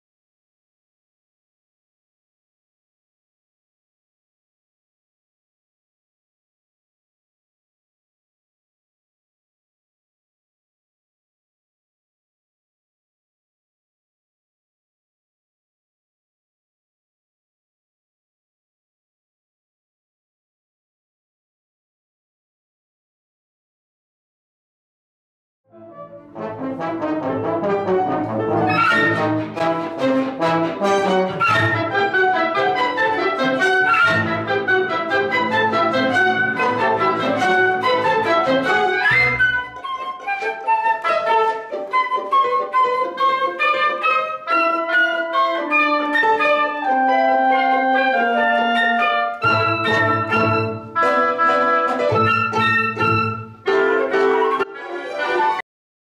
Conductor is .. 指揮是..
May 2022 in Turku Finland.
Orchestra : Panula Academy Orchestra (KIITOS!!)
Teacher: Jorma Panula (KIITOS!!)